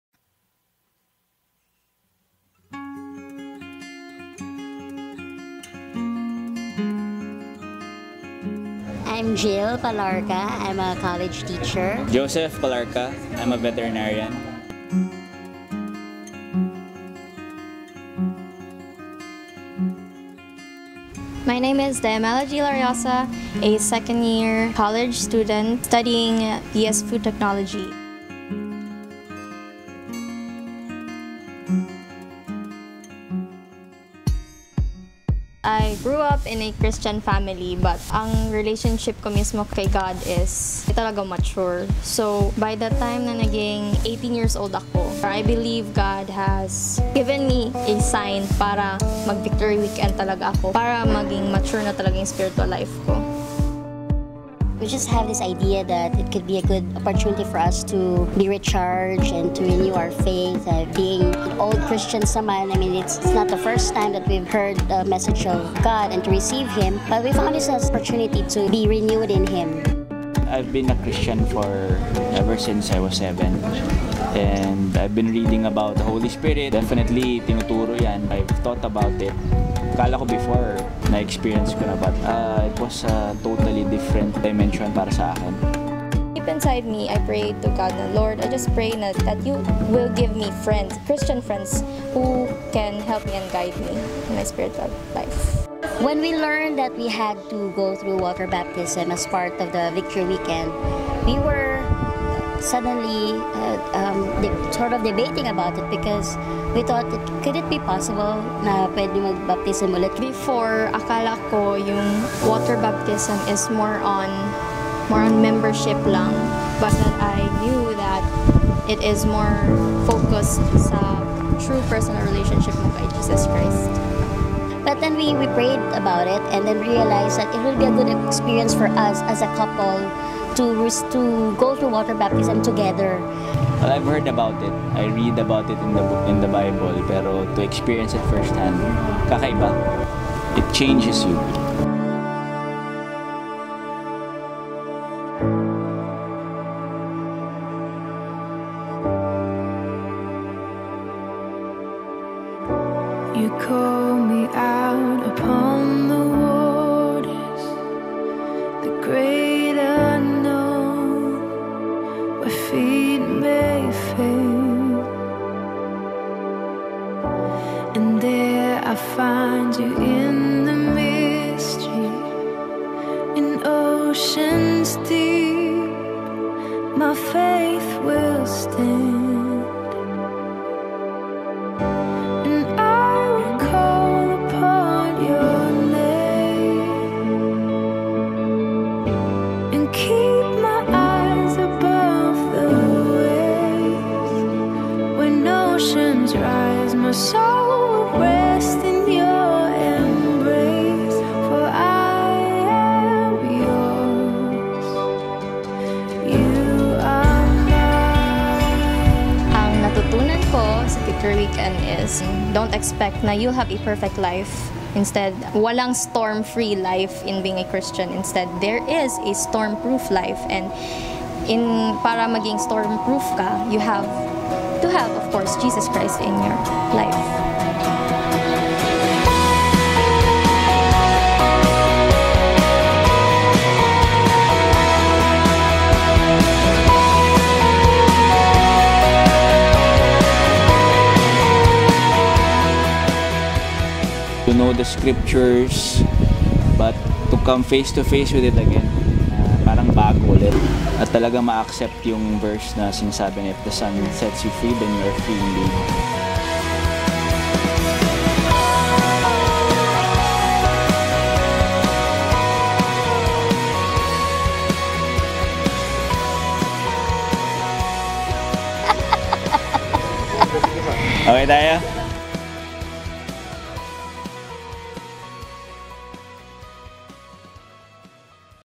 Victory Weekend Feb 2013 Davao
What a special way to celebrate our 5th wedding anniversary -- to renew and strengthen our relationship with the one who fills us with Love -- Jesus Christ :)